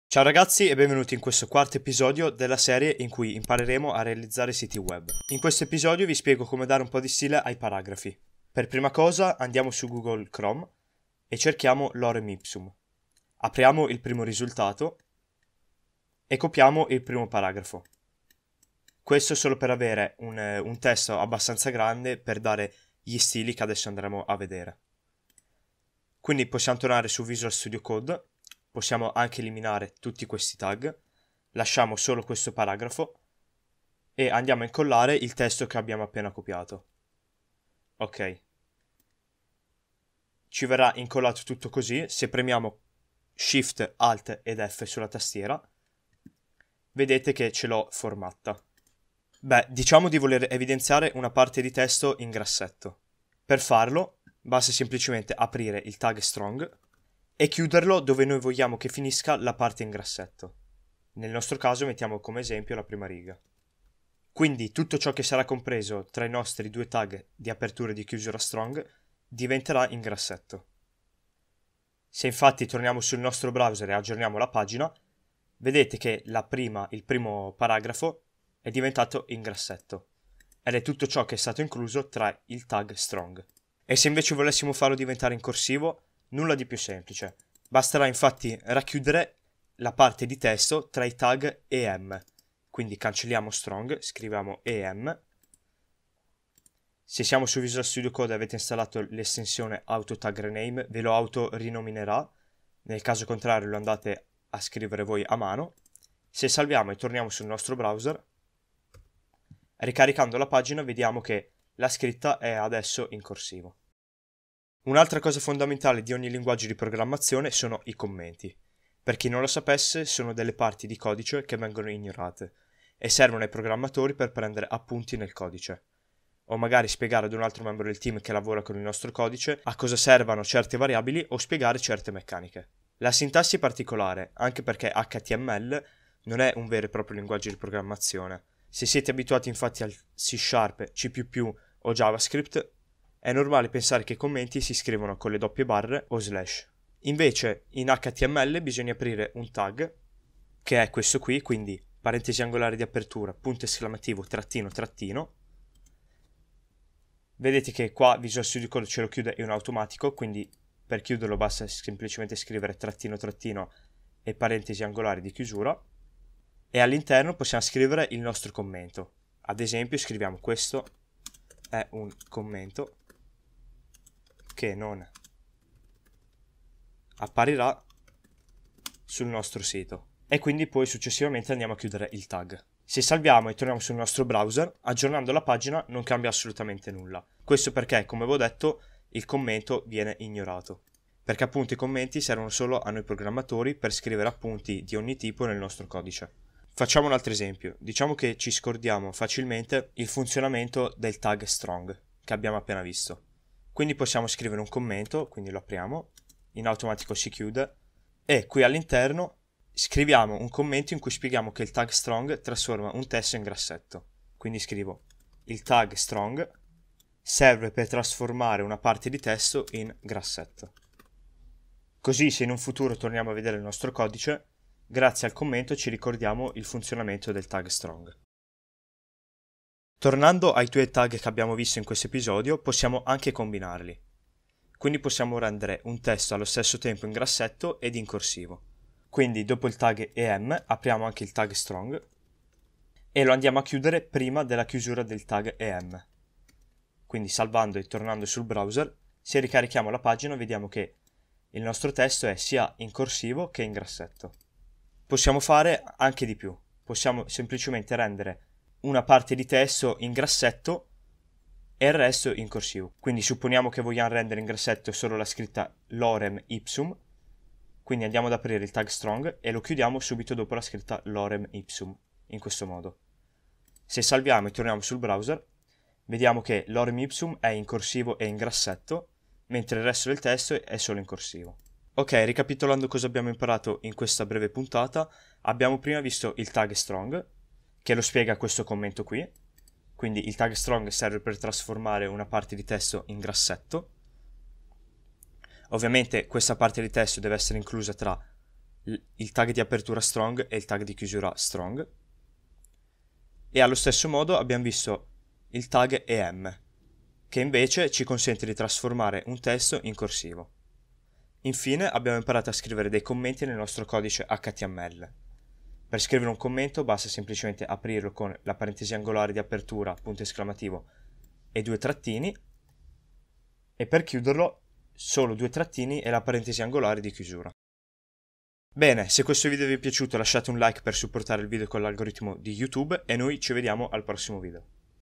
HTML e CSS per Principianti #4 ITA Formattare gli Elementi
Vorresti realizzare Siti Web con HTML e CSS? Sei nel posto giusto, in questa serie si impareranno le basi della creazione di pagine web combinando l'HTML e il CSS, che ti permetteranno di realizzare il tuo primo Sito Web da Zero!

Questo è il quarto video di una serie in cui imparerete a creare Siti Web da Zero!